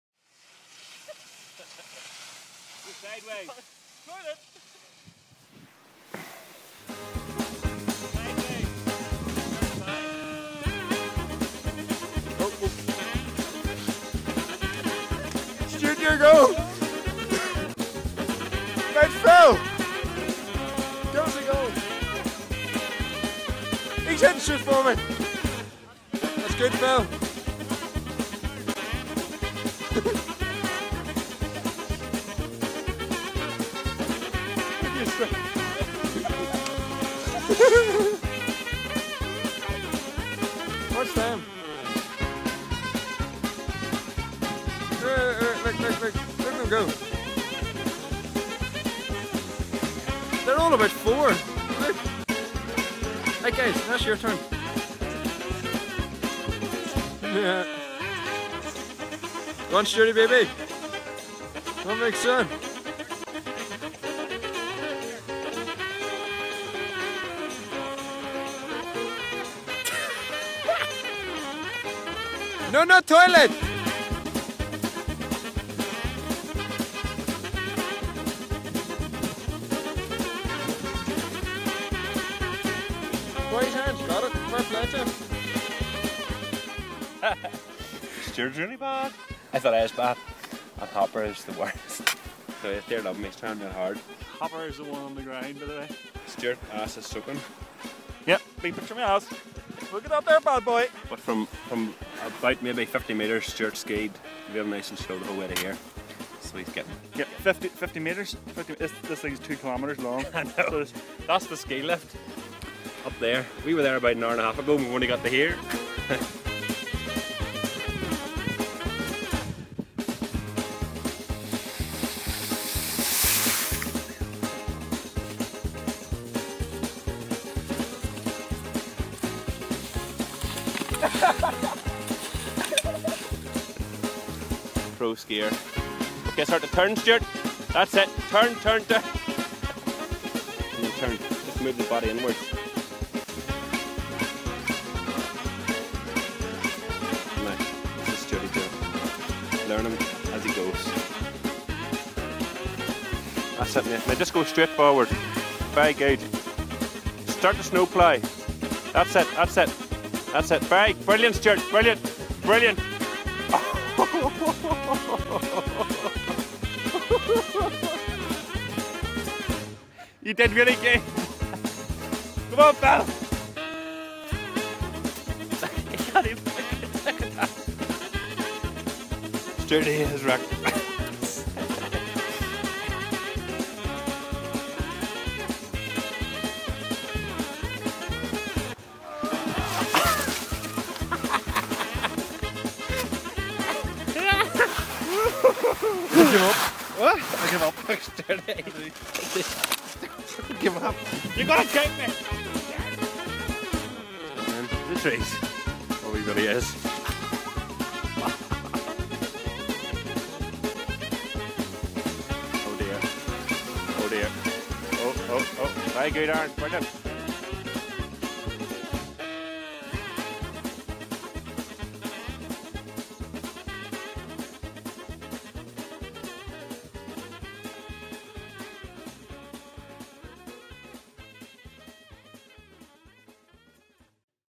BNL & Alpha go Skiing
BNL Productions & Alpa Audio take a day out after the Gaithers gig in Romania to try & ski